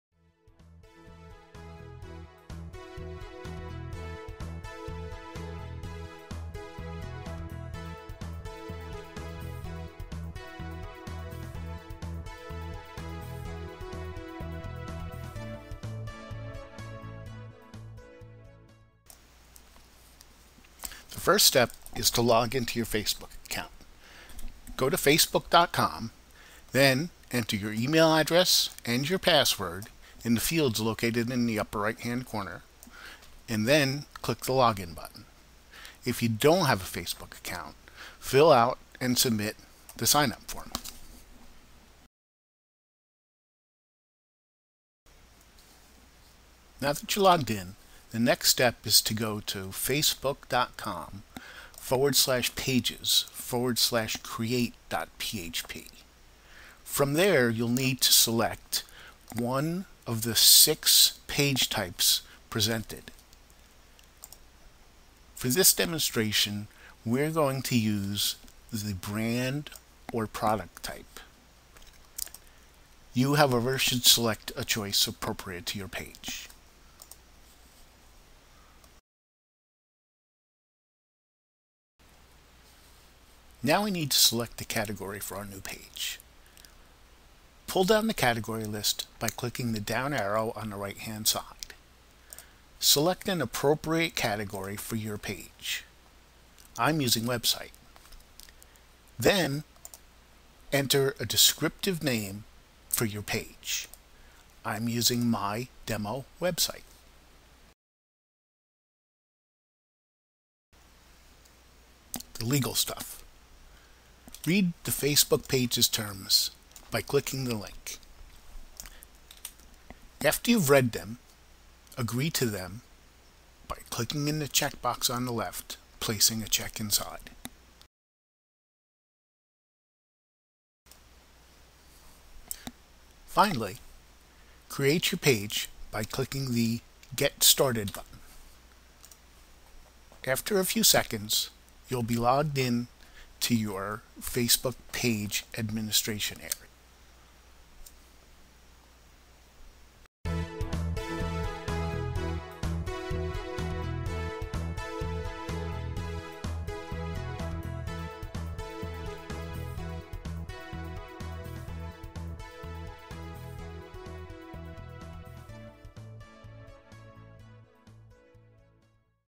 Create your first facebook page in five easy steps and get started in Facebook marketing
- This video demonstrates the basic steps of creating your 1st Facebook page to get you started in Facebook business marketing.